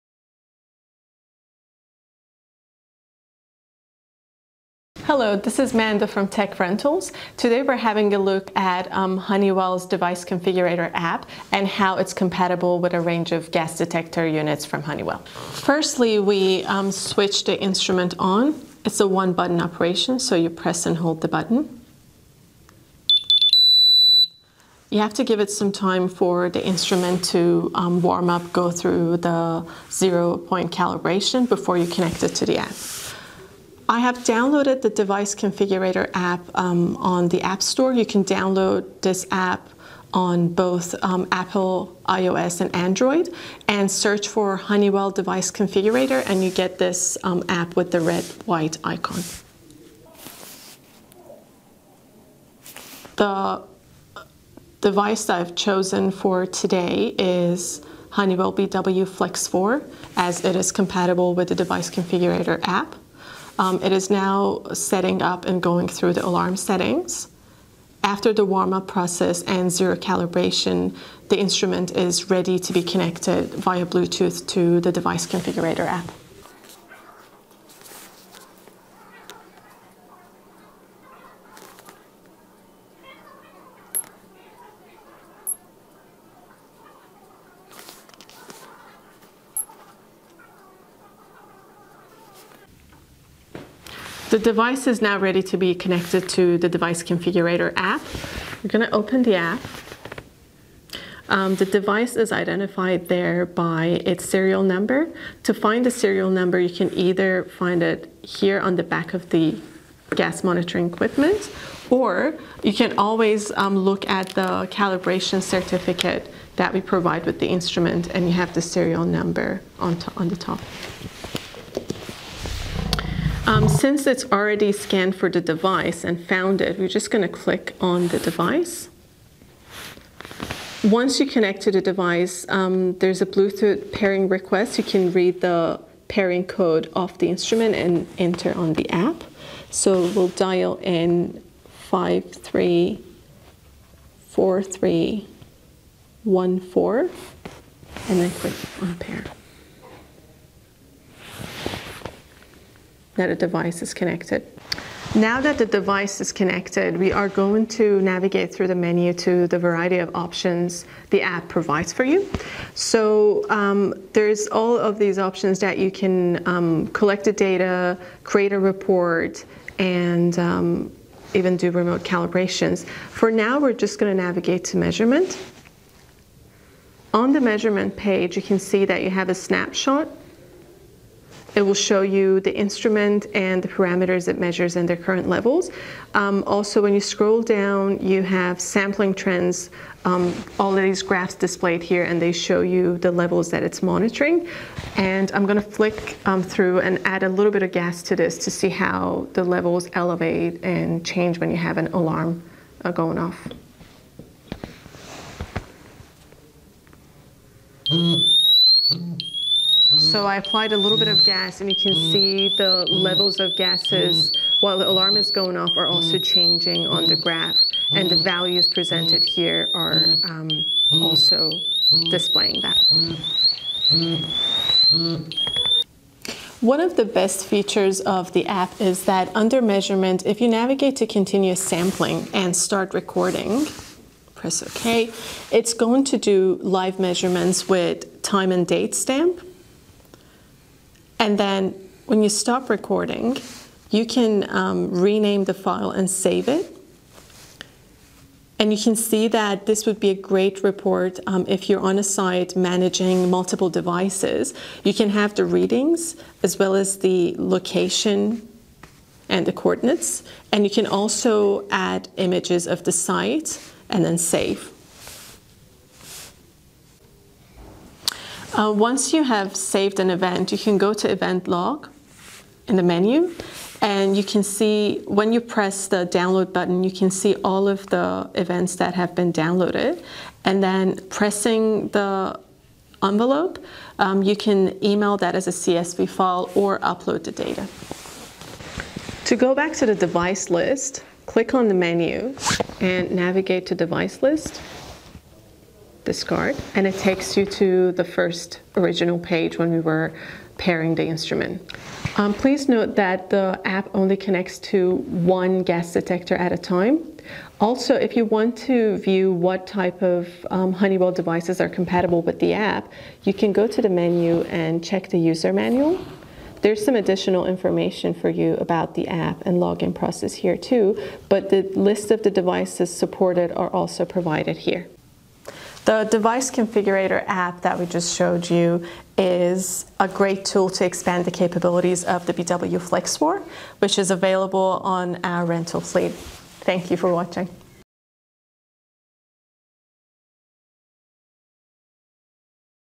Honeywell Device Configurator App
The Honeywell Device Configuration App is a powerful tool for managing your wirelessly enabled BW personal/portable gas detection devices, such as BW Flex4 units; available for rent from TechRentals.

** The app works with smart phones running Android or iOS operating systems and is FREE to download via Google Play Store or Apple App Store.